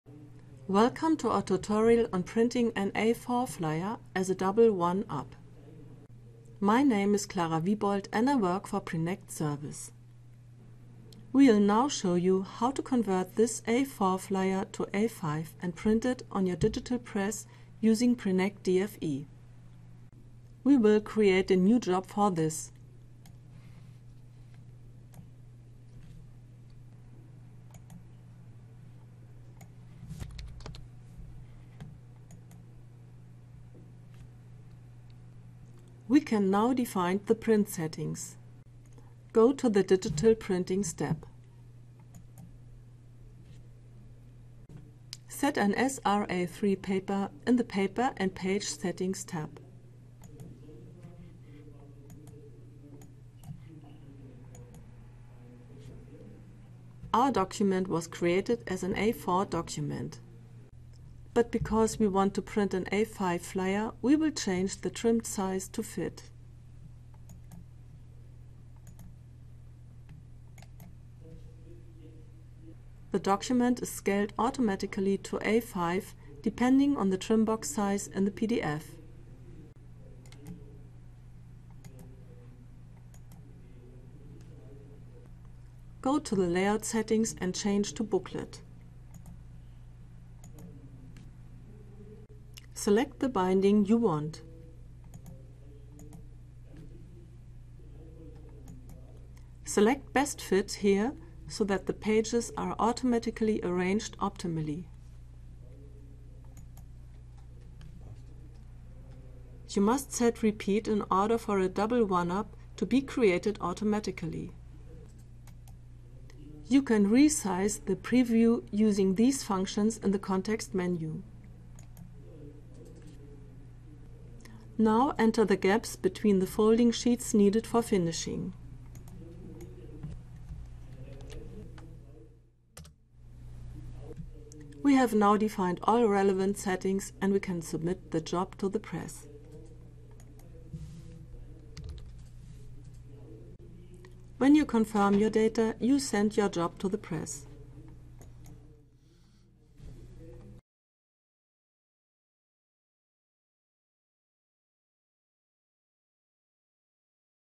2up Brochure Production with Prinect DFE.
Tutorial on printing a A4 flyer as double one-up using the the Prinect Digital Frontend.